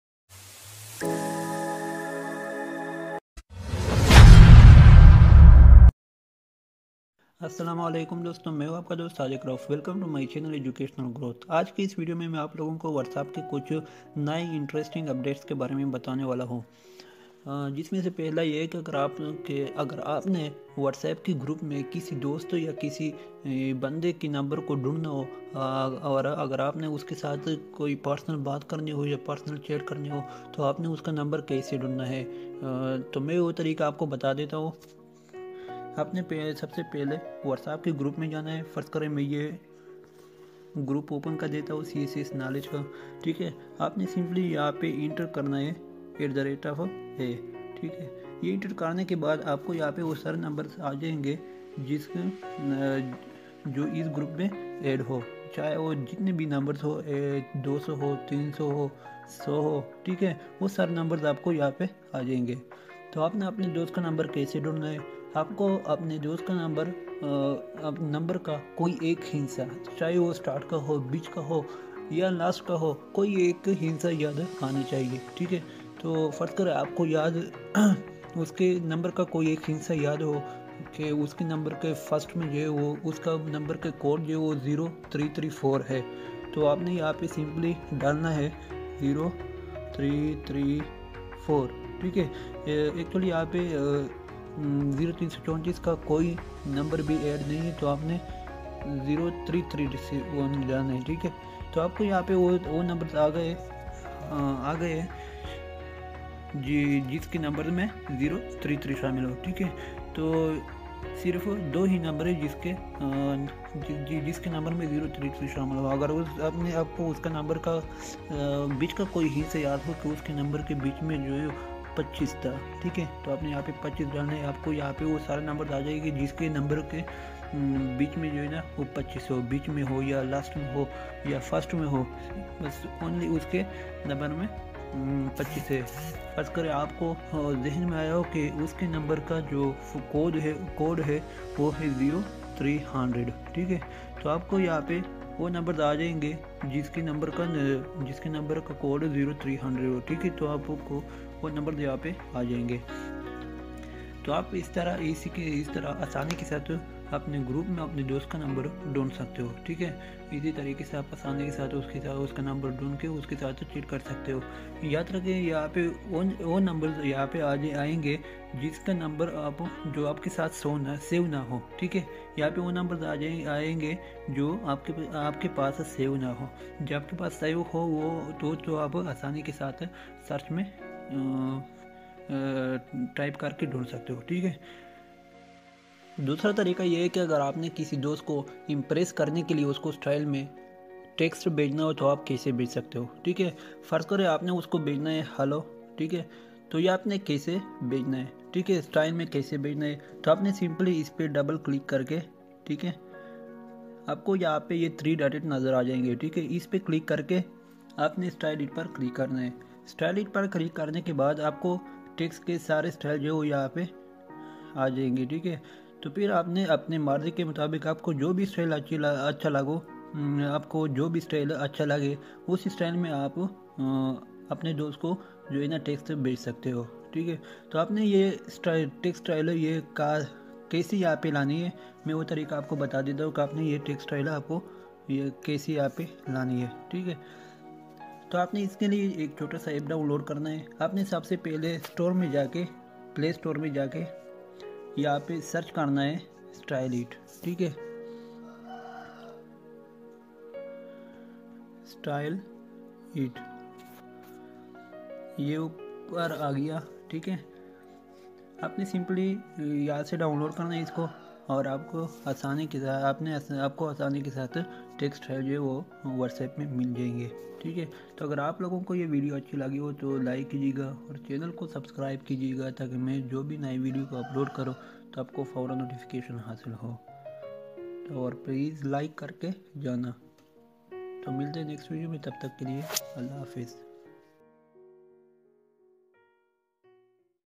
New Amazing Updates of WhatsApp 2019 urdu||hindi
In this video I will discuss about the new Amazing updates of WhatsApp